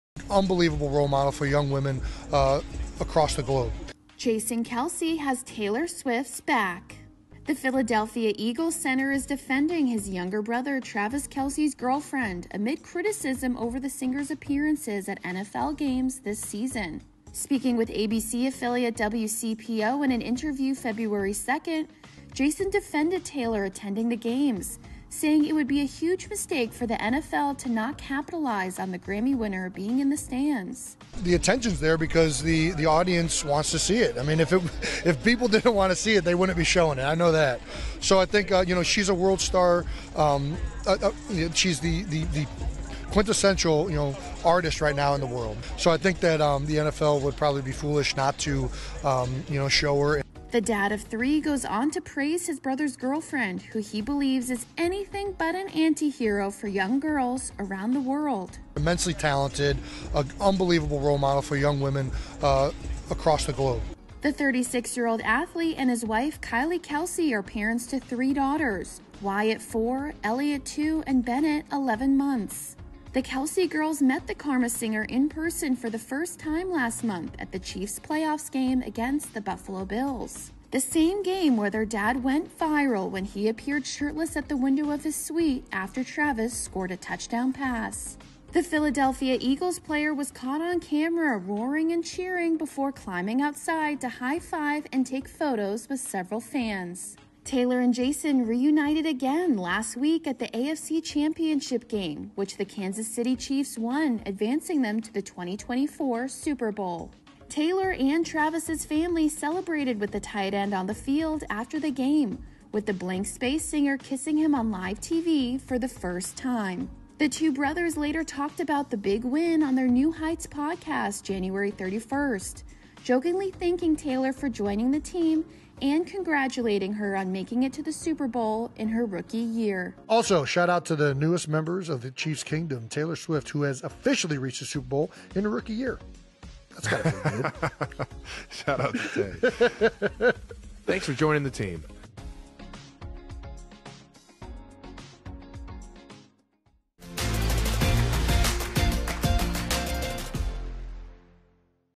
Jason Kelce PRAISES Taylor Swift as “Unbelievable Role Model” for Young Girls | E! News
Jason Kelce is singing the praises of Taylor Swift! In a recent interview, the Philadelphia Eagles center says the NFL should continue to show his brother's girlfriend on tv at the games since she's an "unbelievable role model" for young girls.